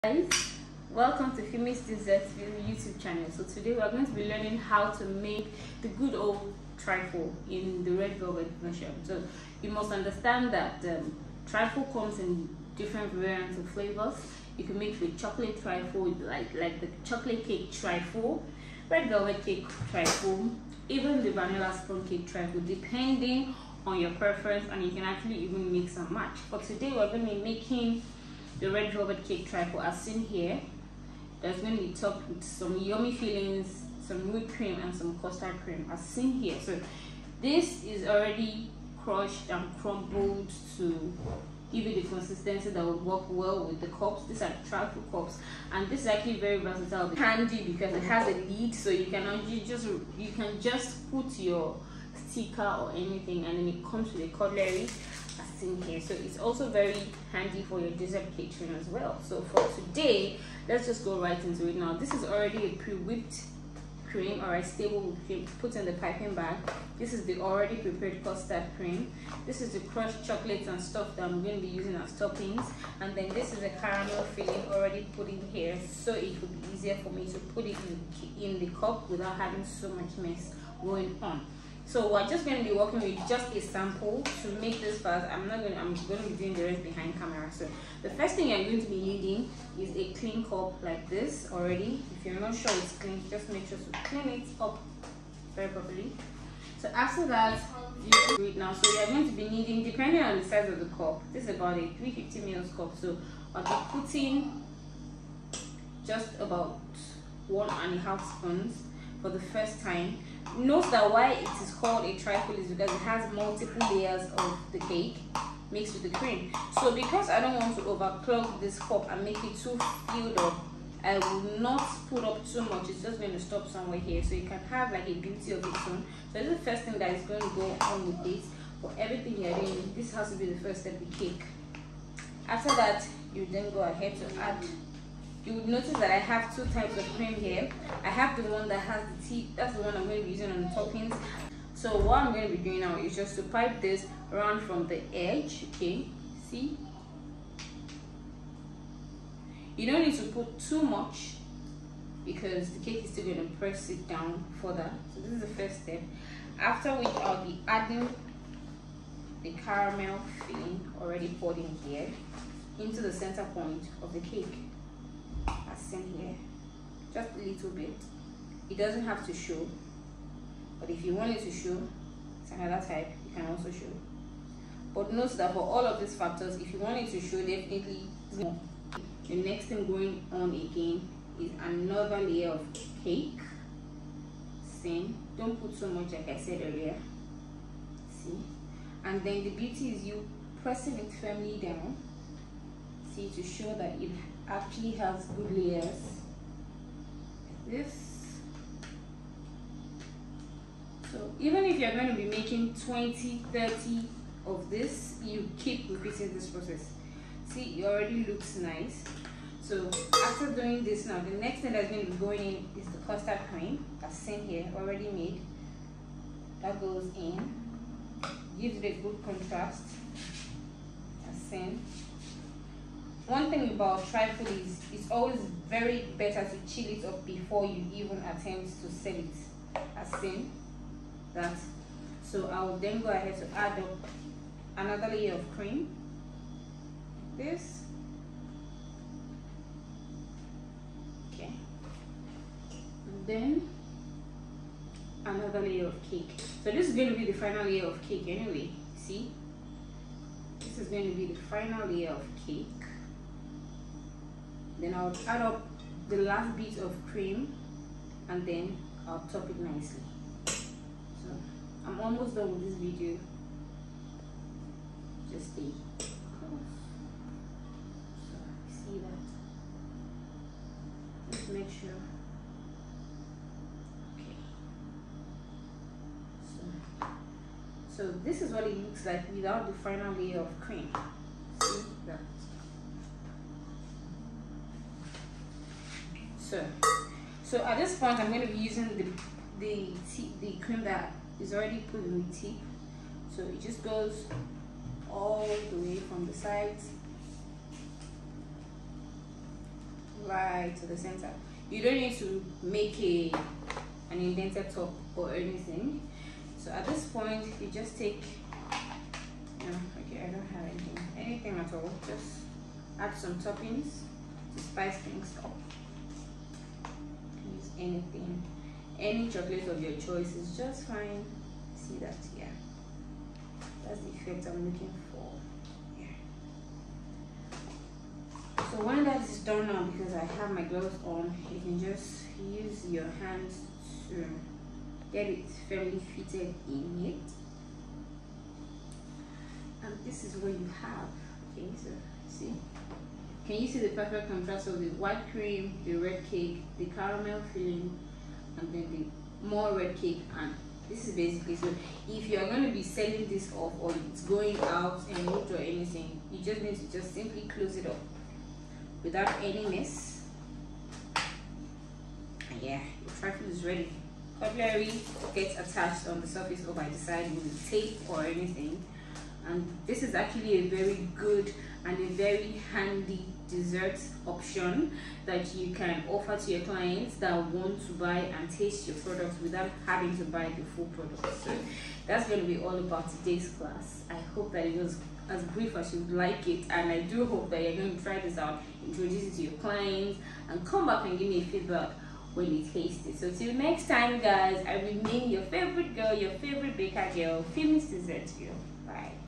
How to make Redvelvet trifle | how to start a trifle desserts business
This is one of the best business idea that i have ever thought off and this particular recipe actually gives me daily money and if you follow my technique properly with a good marketing strategy, trust me the days of saying i am broke is over. listen carefully, sit back, relax and enjoy

TIME CODES

0:00 - INTRODUCTION
0:54 - TRIFFLE CUPS FOR DESSERTS
1:10 - RECIPE
1:43 - MAKING OF REDVELVET TRIFFLE DESSERTS
2:25 -TIME TO ADD TE WHIPPED CREAM
2:49 - ADD A LITTLE CUSTARD
4:13 - HOW THE DESSERTS LOOKS LIKE